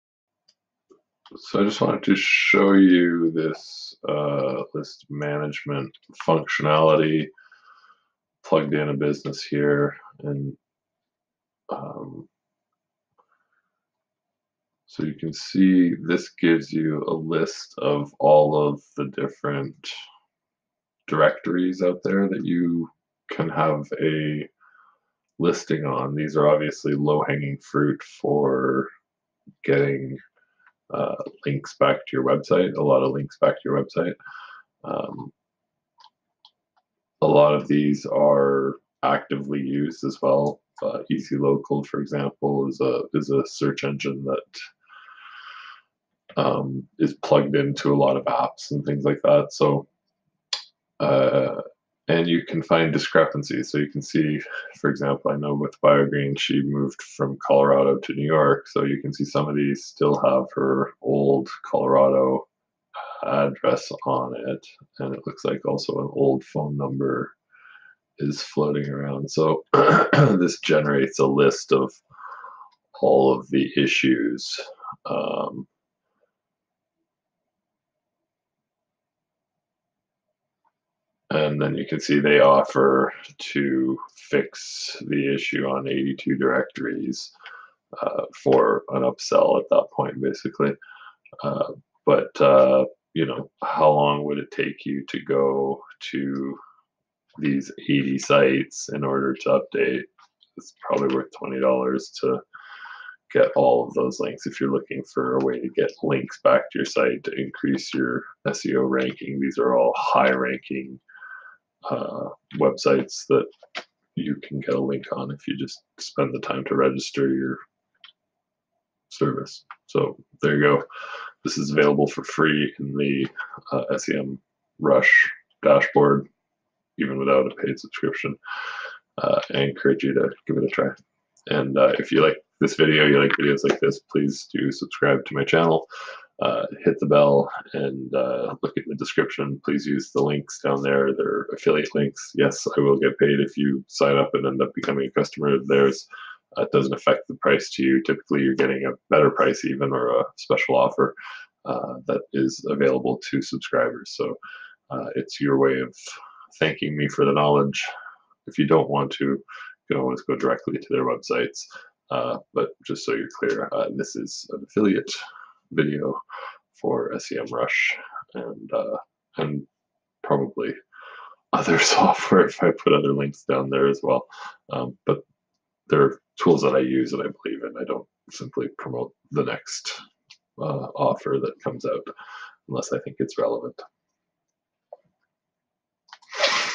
How to check your directory listing references using SEMrush
You can use their directory search feature for free to see how many places you are missing out on. Use that info to manually register them, or pay for the service to be done for you.